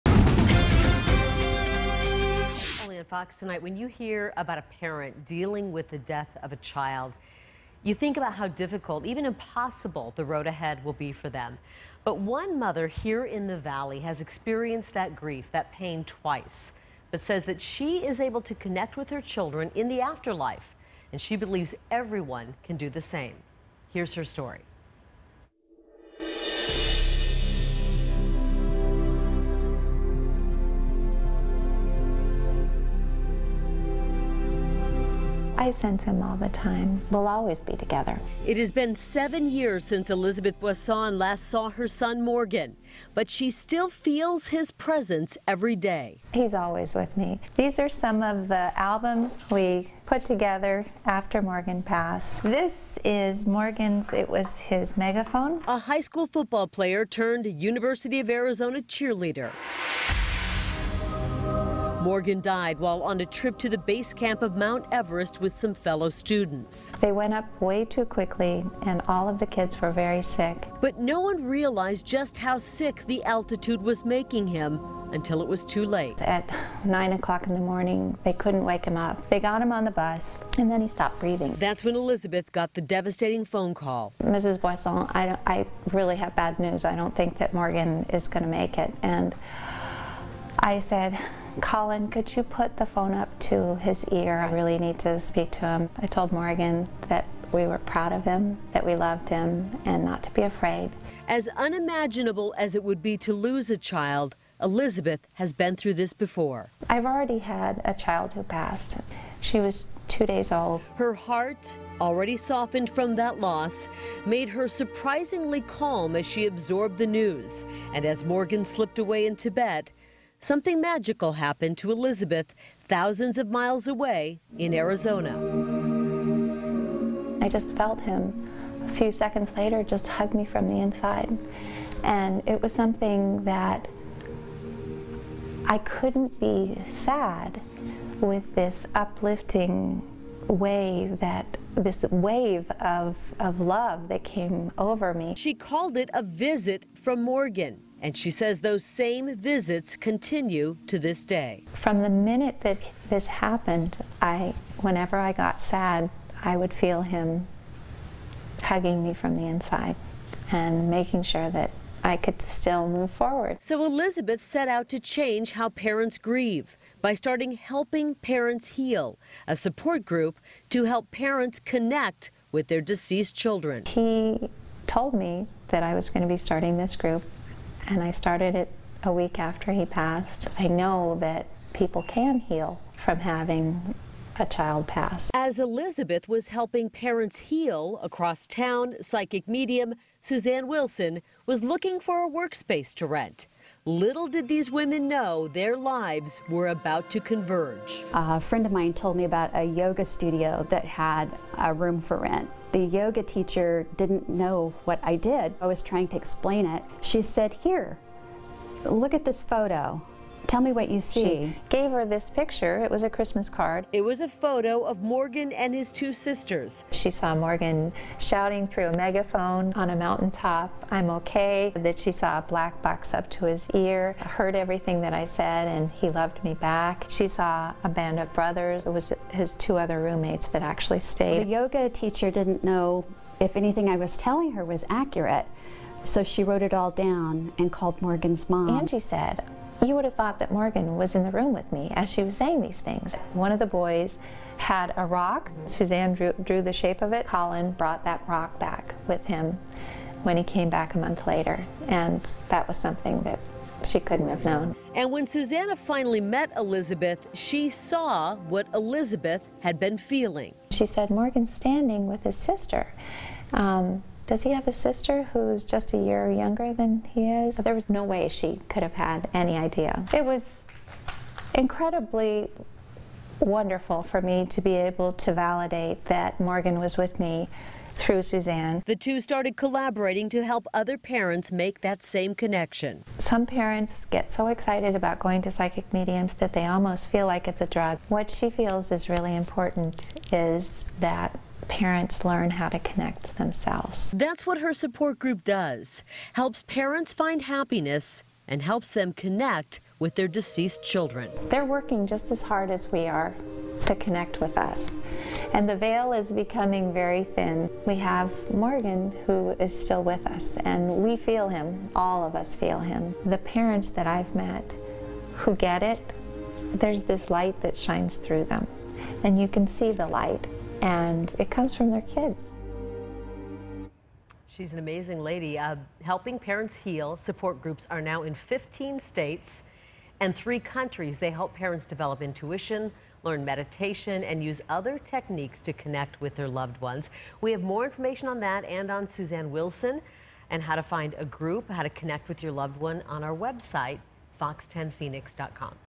Helping Parents Heal: How a valley mom connects with her children in the afterlife
A mother who lost her son says she still feels his presence every day and along with a psychic medium, she's helping other grieving parents get reassurance from beyond. FOX 10's Kari Lake reports.